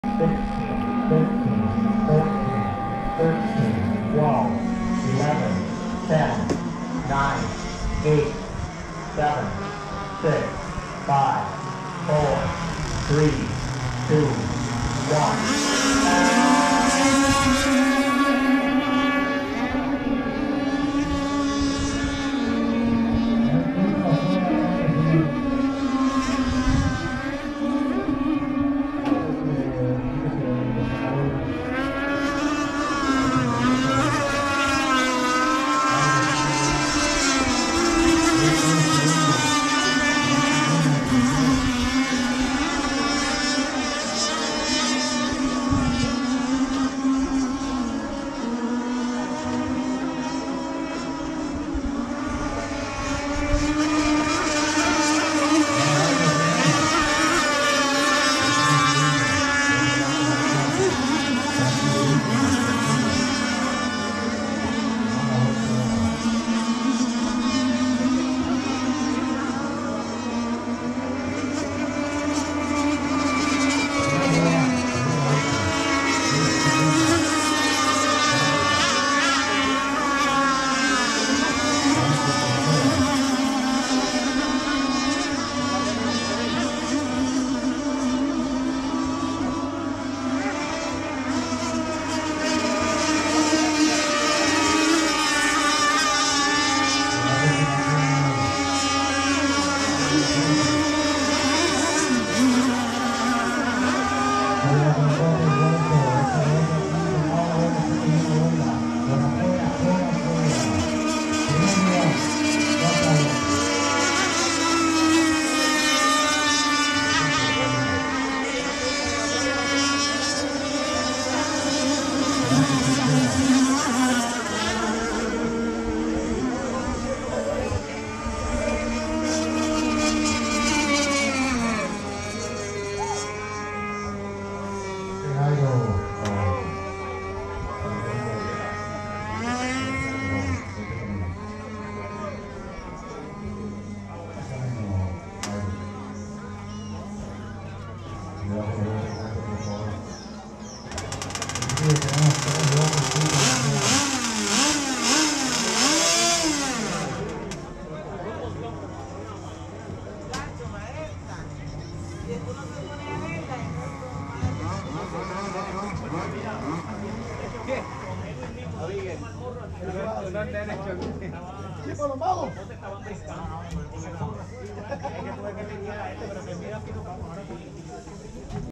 Superboat heat - organizado por Loko lake en Lago dos Bocas - Utuado, Puerto Rico.
Superboat tercer heat - 4ta carrera circuito Botes a control remoto de Puerto Rico organizado por Loko lake  Loko Lake RC Boats  en Lago dos Bocas en Utuado. Evento pro fondos Aliaza Autismo de PR.